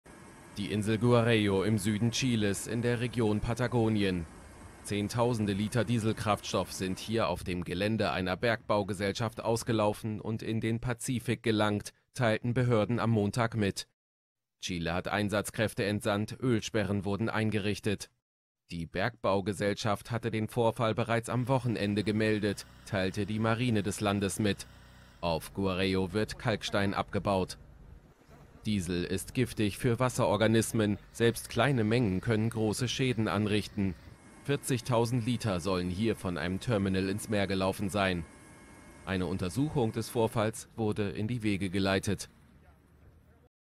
Insel Guarello in Chile: Zehntausende Liter Diesel verschmutzen Pazifik
Die Bergbaugesellschaft hatte den Vorfall bereits am Wochenende gemeldet, teilte die Marine des Landes mit. Auf Guarello wird Kalkstein abgebaut.